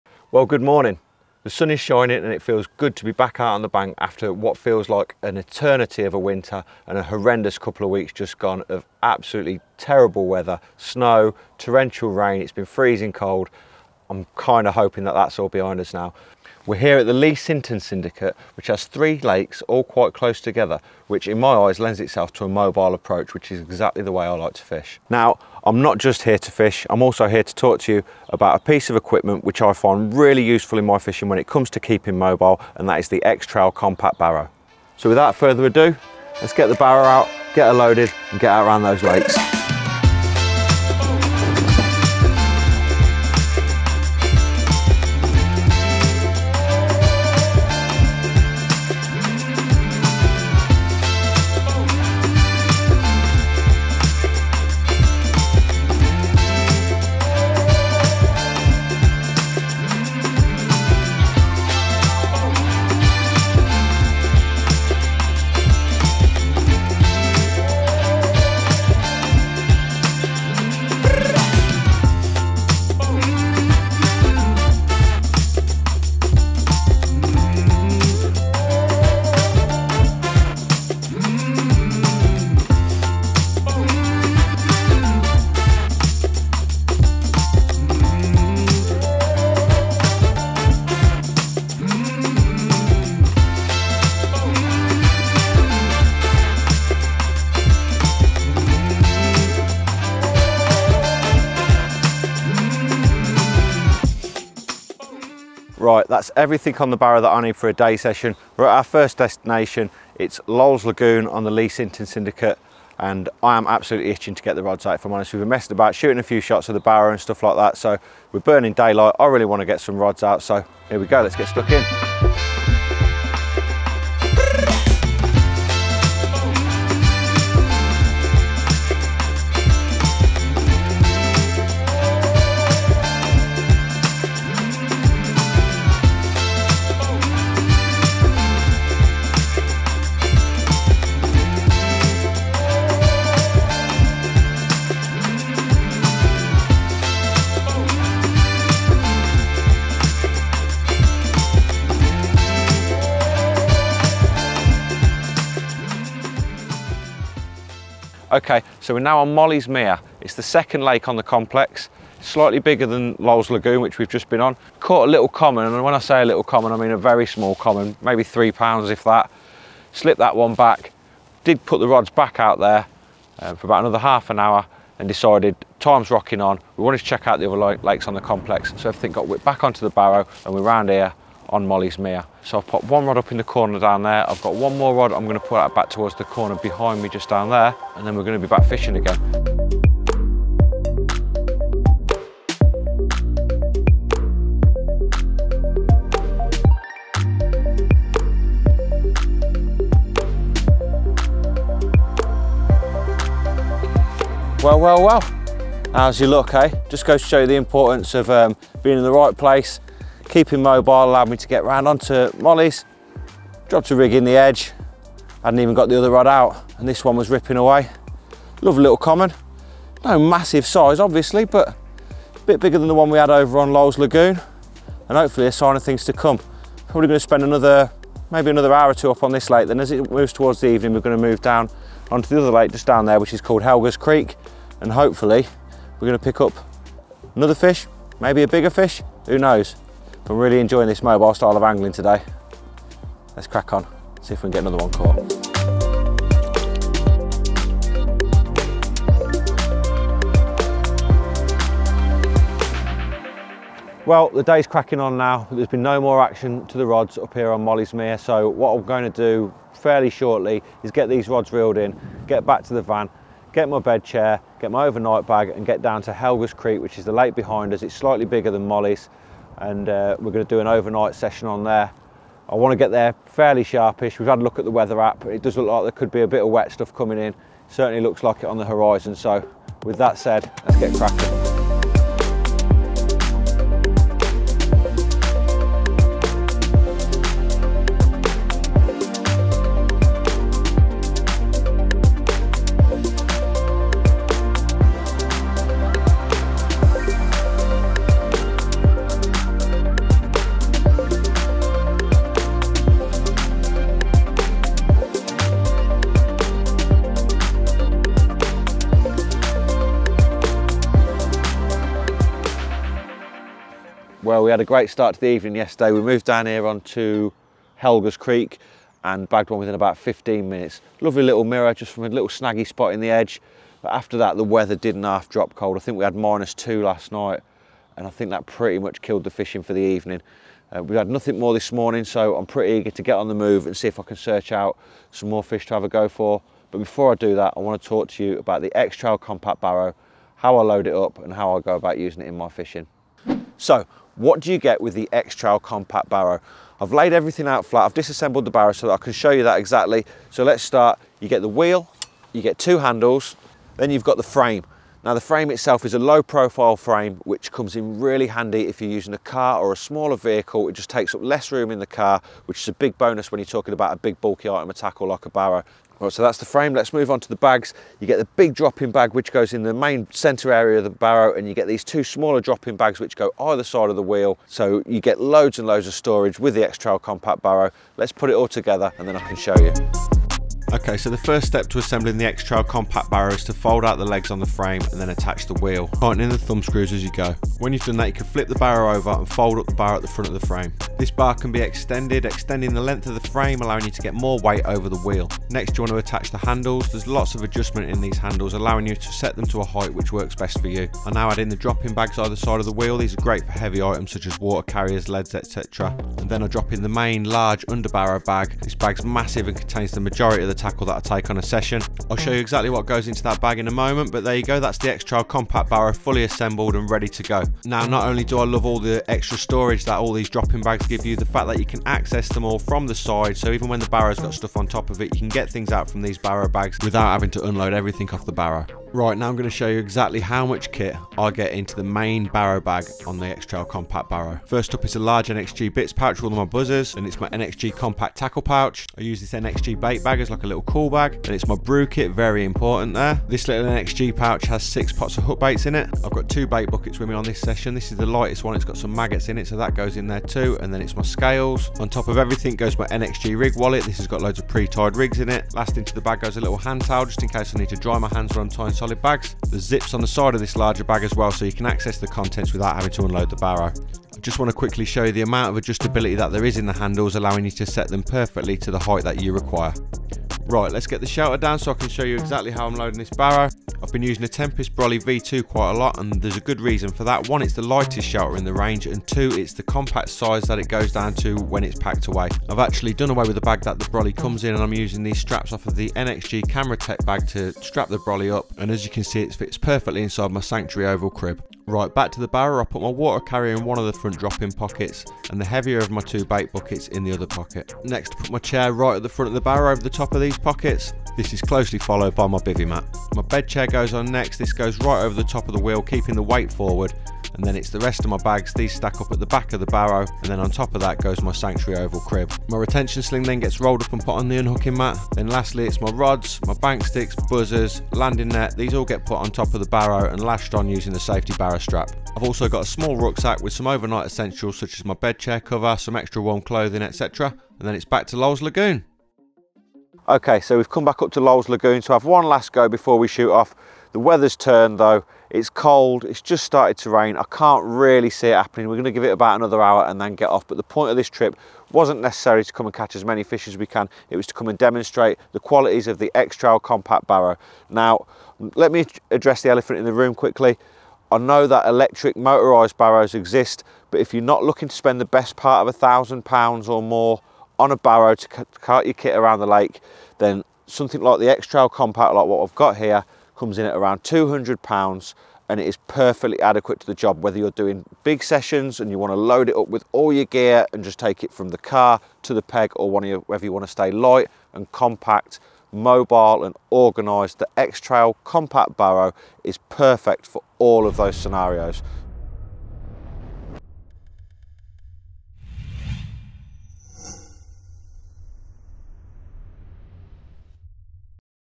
Mobile Carp Fishing with the Trakker X-Trail Compact Barrow
Staying mobile can be a massive edge when it comes to targeting carp at any time of year. In this video, Team Trakker's Gaz Hood – armed with his trusty X-Trail Compact Barrow – visits Arrow Meadow Fishery's Leigh Sinton syndicate which offers specimen carp fishing across three lakes, and employs a mobile approach to get around and fish all of the waters in less than 24 hours.